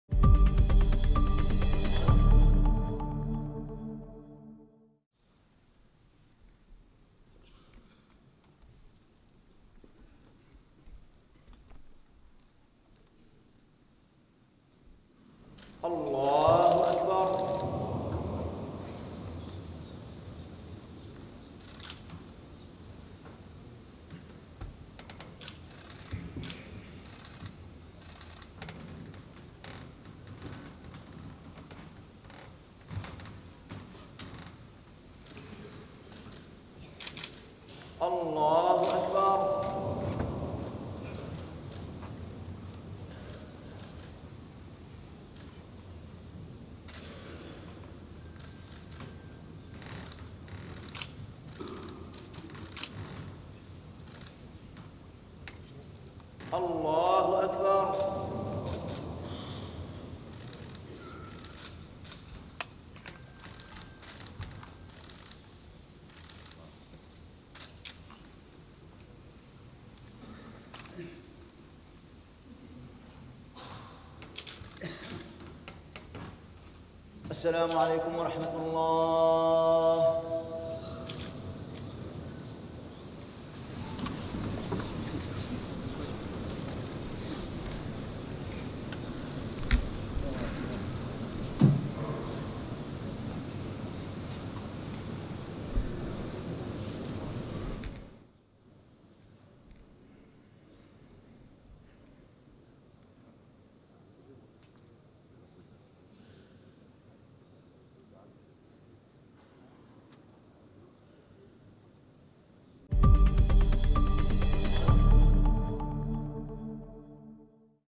Funeral prayer for Lt. General Khamis Mattar Al Mazeina
Hundreds of well-wishers gathered at the Zabeel Mosque to attend the funeral prayer for Lt. General Khamis Mattar Al Mazeina, Commander-in-Chief of Dubai Police, who died on Thursday night.
The funeral prayer was held after the Friday prayers. The late Dubai Police Chief was then laid to rest in Dubai's Al Qusais graveyard.